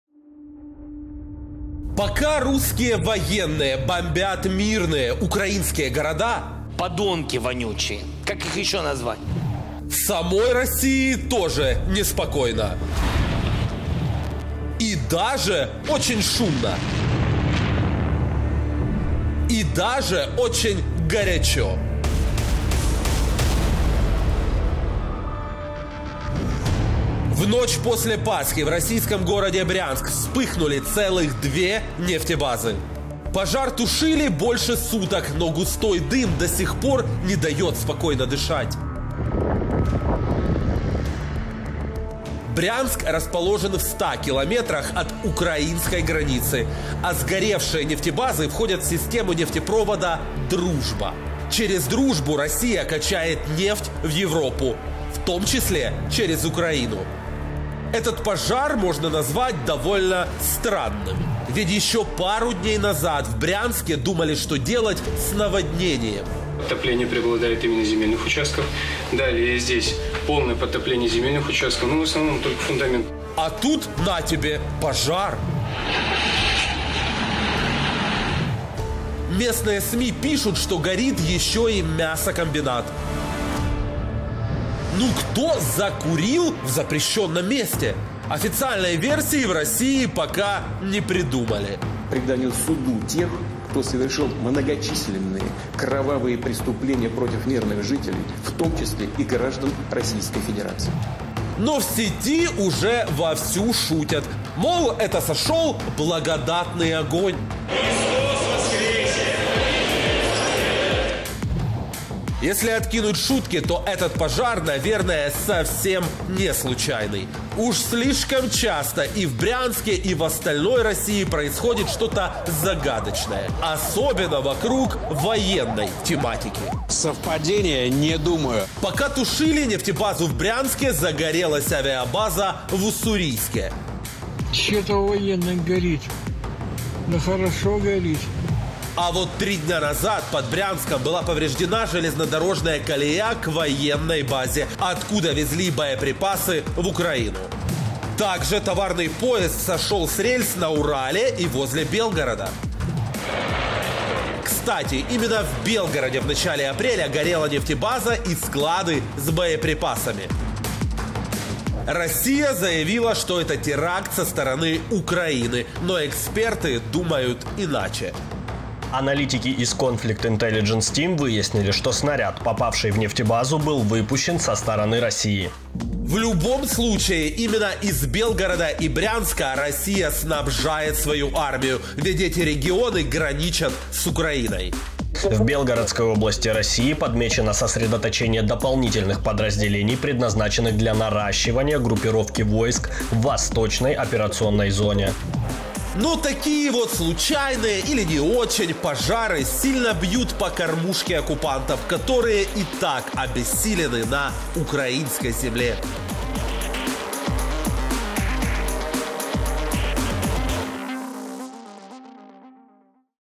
Диверсійна діяльність на території Росії
Вибухи на нафтобазах і залізничних дорогах в Росії. Хто та як це робить. Військовий експерт про диверсійну діяльність на території Росії.